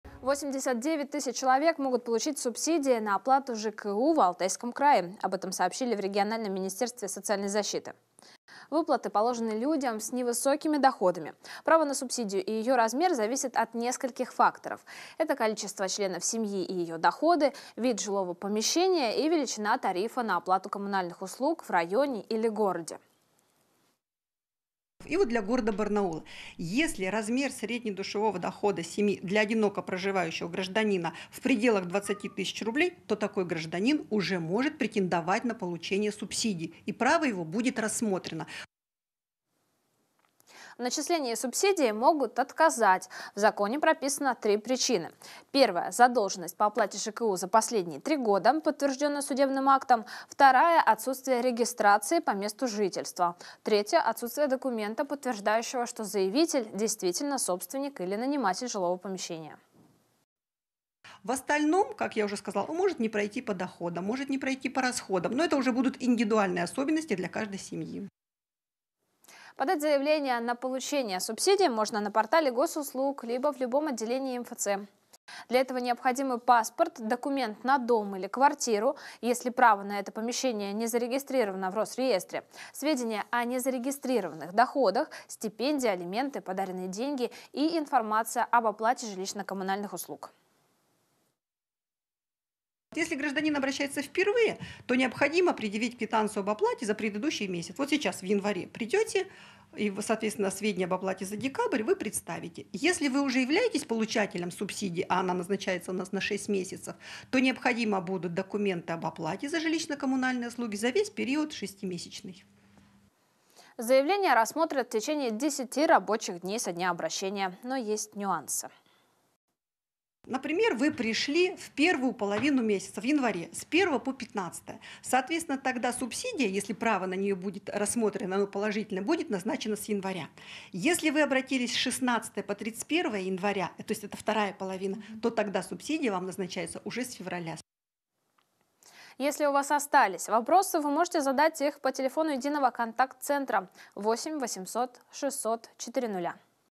89 тысяч человек могут получить субсидии на оплату ЖКУ в Алтайском крае
Право на субсидию и ее размер зависят от нескольких факторов. Это количество членов семьи и ее доходы, вид жилого помещения, величина тарифа на оплату коммунальных услуг в районе или городе.